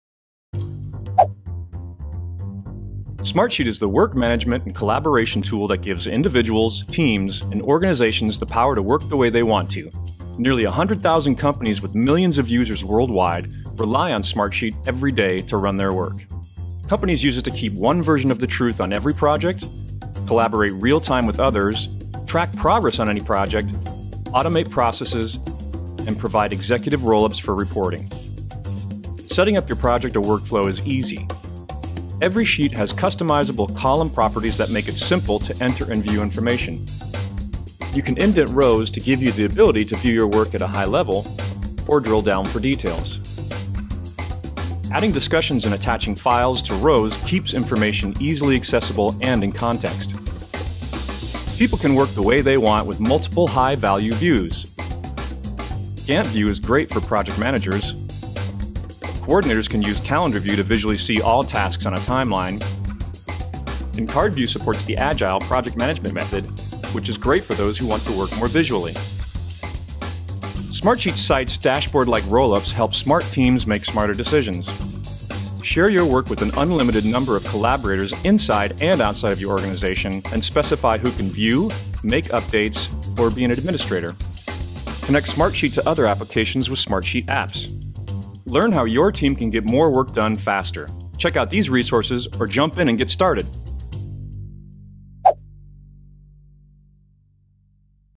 Work Your Way with Smartsheet (Malaysia)
Smartsheet. Work Better.
Manage and automate collaborative work
with a platform trusted by the
world’s most productive companies.

How these companies and organizations are able to work Smarter?
How to track team's work progress effectively and maximize performance and results?

Is there a new way to simplify and streamline your existing business operations and processes other than Google Doc or Microsoft Office Doc and any other Business Suites?